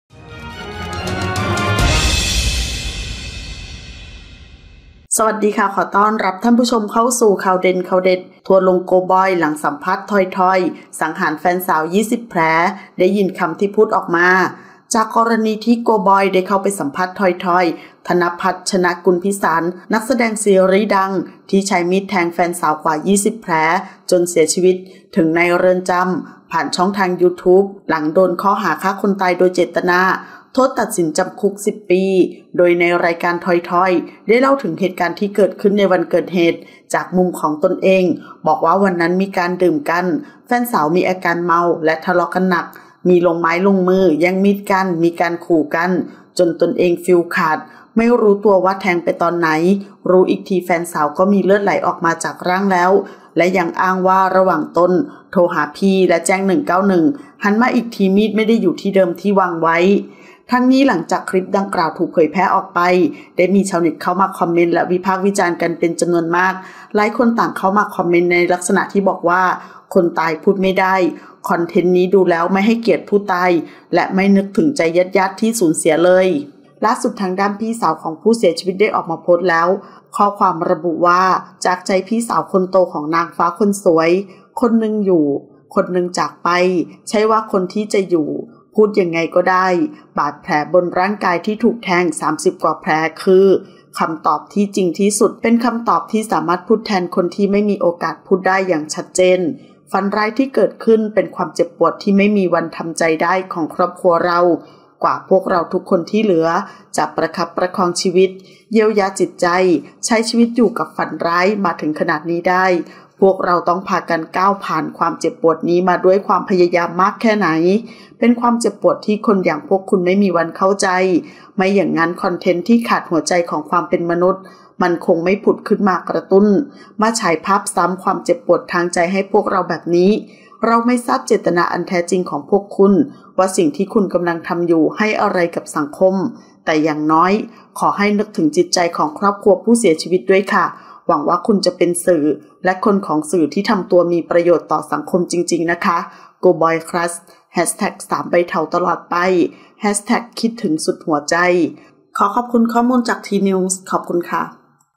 ทัวร์ลง “โกบอย” หลังสัมภาษณ์ “ทอยทอย” สังหารแฟนสาว 20 แผล ได้ยินคำที่พูดออกมา
00:00 - Intro
00:10 - News
01:00 - Minute